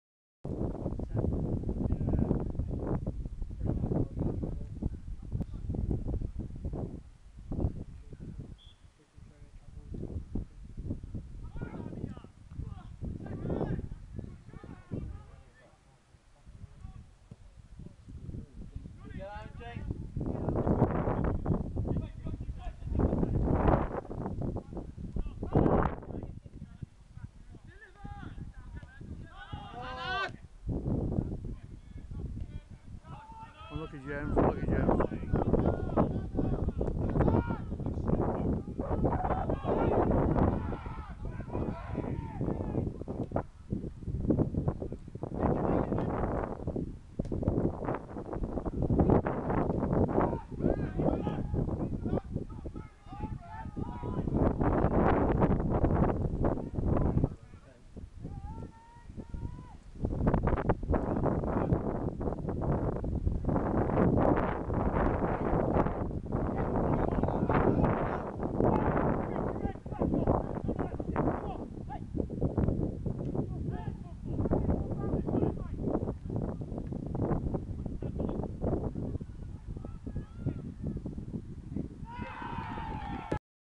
Duchy Div 3 game, Lamerton com res 2 v 1 St Stephen Res
match highlights Lamerton score on 27 minutes goal scored by Daryl Panter.